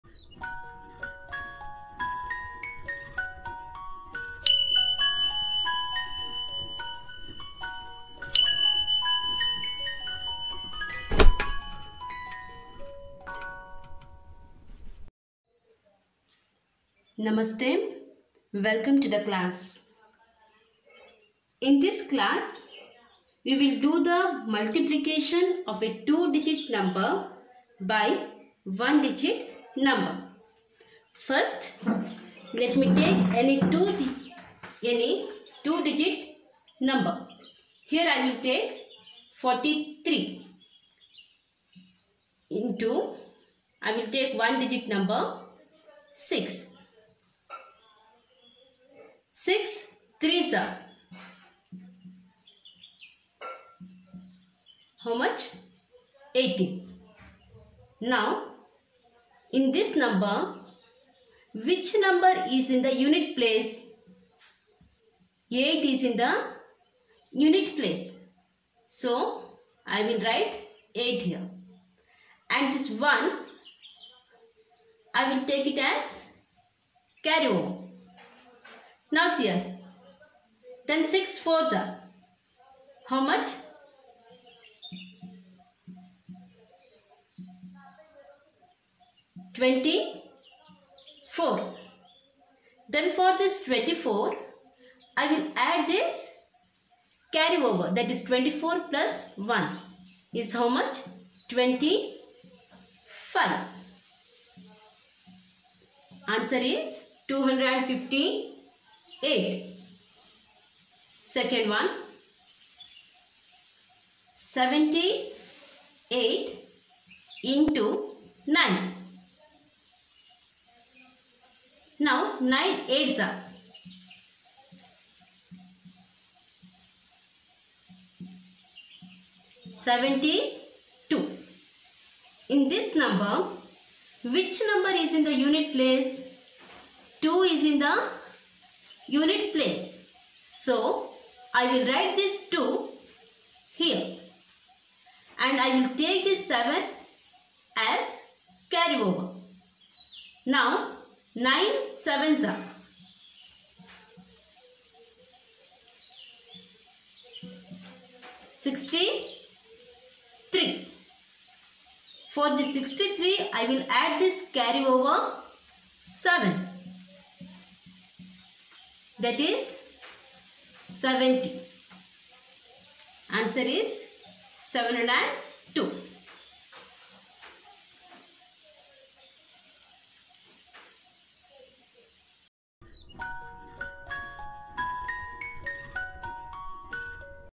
Concept -03: Multiplication of 2 digit number by 1 digit number
Multiplication of 2-digit number by 1 digit number.
In this video I have explained the Concept of
multiplication step by step.It is helpful for the beginners who are learning the Concept of Multiplication.